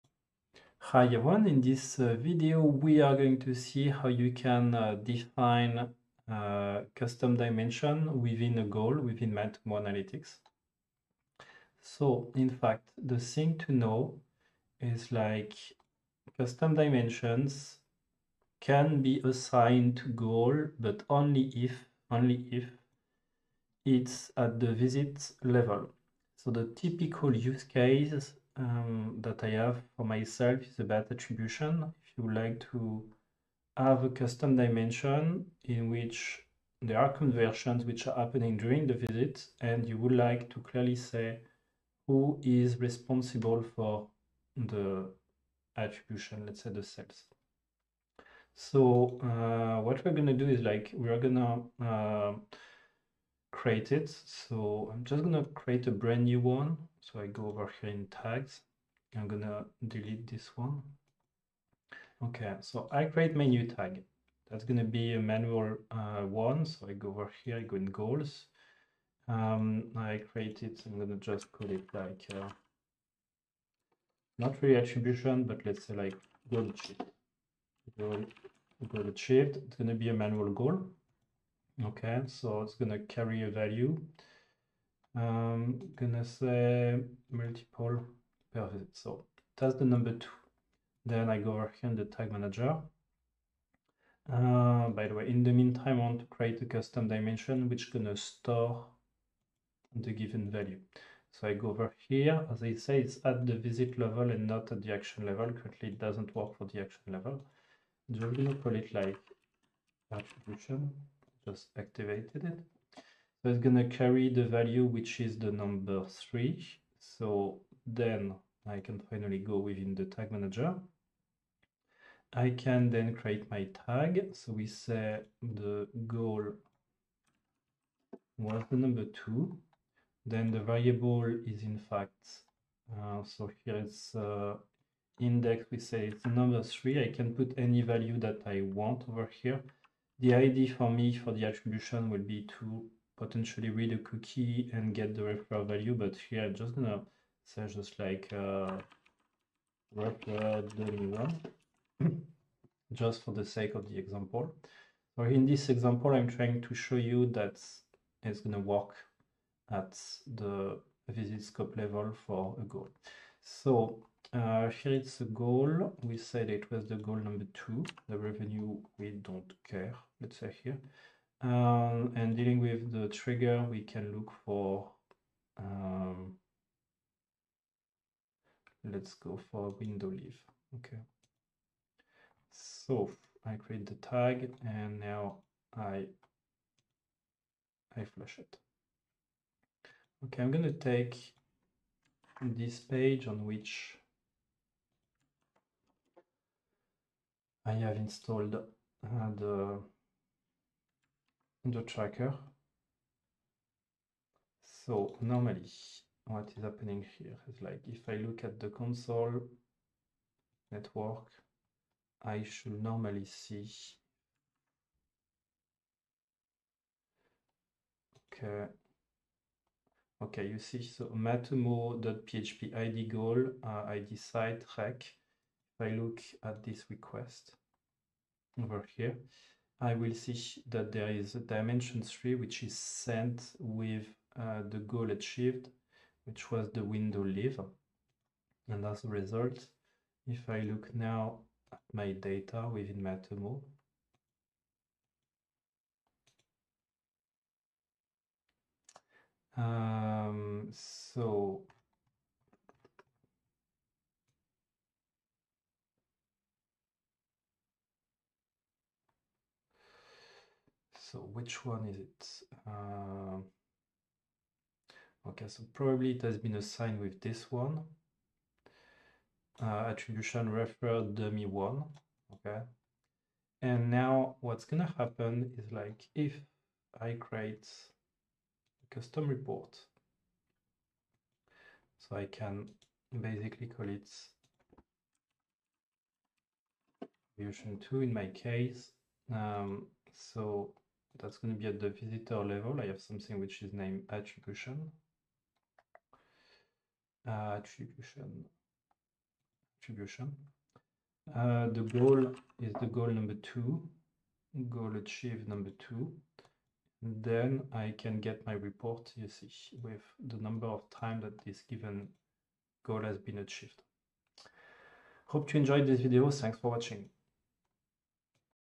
How to define custom dimension at the goal level in Matomo Analytics?
Custom dimensions can be assigned to Goals within Matomo as far as you assign them at the visit scope level.